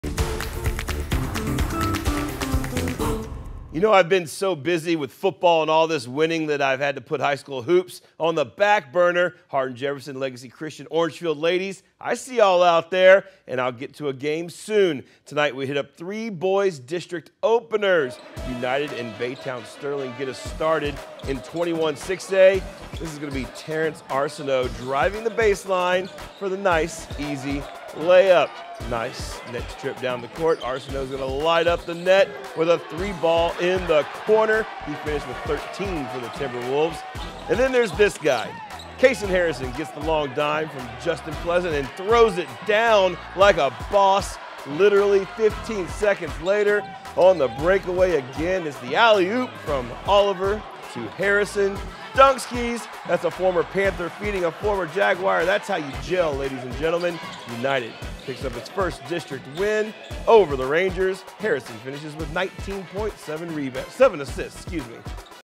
Harrison leads United past Sterling 72-45
Check out a pair of dunks by Kasen Harrison as he lead the Timberwolves to their first district win.